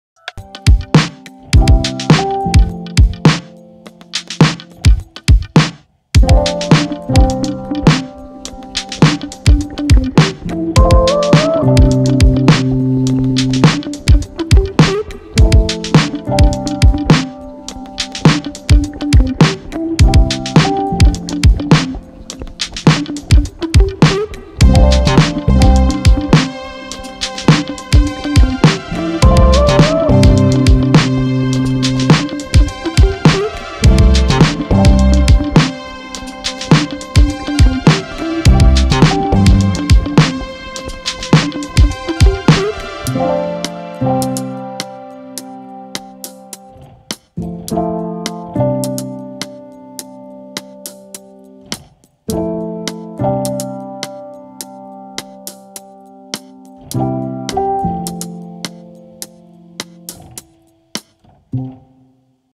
Paper art snail
how to make paper snai.in this video you will learn to make paper snail.you will like the idea which is very easy n simple.thanks